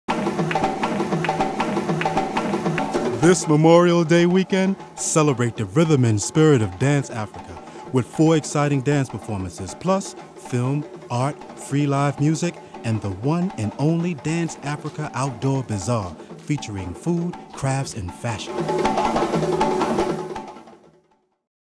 DanceAfrica 2009 (Spring 2009)
May 22 at 7:30pm
May 23 at 2 & 7:30pm
May 24 & 25 at 3pm

Rhythmic Reflection: African Legacies Revealed
Artistic Director Chuck Davis

Evidence, A Dance Company (Brooklyn)
Farafina Kan (Washington D.C.)
The SeeWe African Dance Company (NY)
and BAM/Restoration DanceAfrica Ensemble (Brooklyn)

Now in its 32nd year, DanceAfrica, guided by the inspired, creative vision of Artistic Director Chuck Davis, unites dance companies from across the nation to shake the Opera House stage. This year Washington DC-based percussion orchestra Farafina Kan—the "sound of Africa"—shares the history and spirit of West African music; New York group SeeWe African Dance Company uses African rhythms and instruments to enlighten audiences; Brooklyn-based Evidence, A Dance Company fuses traditional African dance with contemporary movements and spoken word; and the BAM/Restoration DanceAfrica Ensemble from Bed-Stuy performs for the thirteenth straight year at BAM. A Memorial Day weekend tradition, DanceAfrica is packed with dance, music, art, and film events—plus the one-and-only DanceAfrica outdoor bazaar.